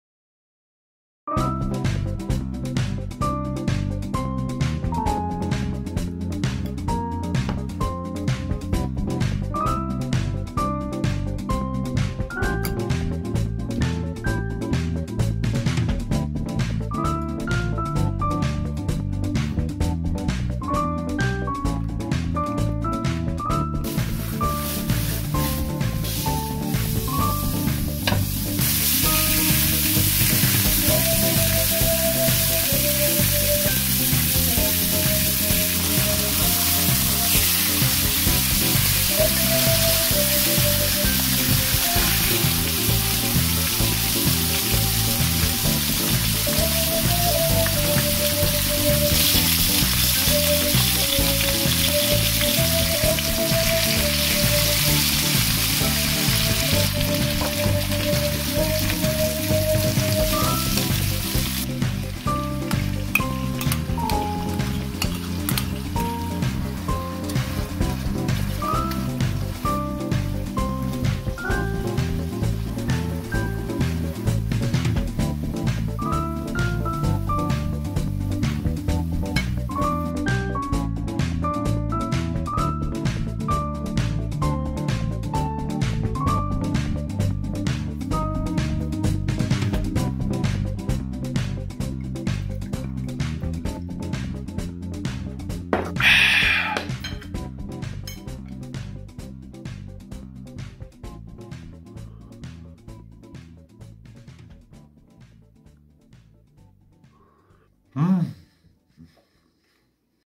매콤 달달 감칠맛 폭발 멈출수 없는 나의 젓가락질을 멈추게 해줘 삼겹살 집에서 맛있게 먹는 법 그리고 최고의 술안주
집에 삼겹살은 있는데 오늘따라
삼겹살을 다르게 먹고 싶다?
그렇다면 한국인의 고추장과 조화를 이룬
고추장 삼겹살에 소주한잔 어떠신가요?